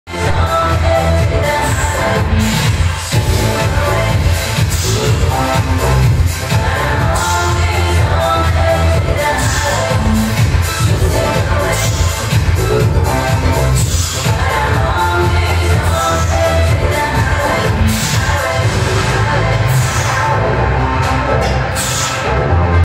Cvičenie na hyperextenzná lavička - 2. časť
Cvičenie brucha a chrbta patrí medzi najdôležitejšie cvičenia a nikdy by ste ich nemali zanedbávať. Príslušné svaly chrbta a brucha by sa mali trénovať tak, aby vytvorili rovnováhu a protiváhu.